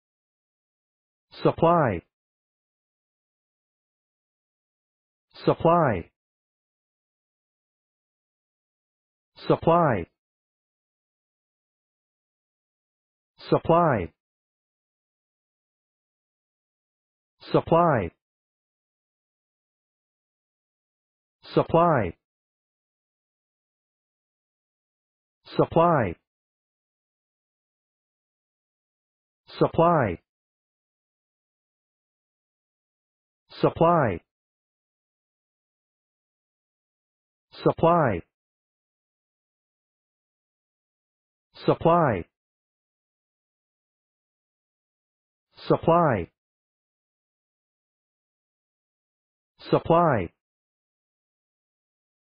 Pronunciation of Supply | Definition of Supply
Supply pronunciation | How to pronounce Supply in English?
/sə`plaɪ/
Meaning of Supply | What is Supply?
(1) (verb) circulate or distribute or equip with; (2) (verb) give something useful or necessary to; (3) (verb) state or say further; (4) (verb) give what is desired or needed, especially support, food or sustenance; (5) (noun) the activity of supplying or providing something; (6) (noun) an amount of something available for use; (7) (noun) offering goods and services for sale;
What is Supply | Supply meaning and definition?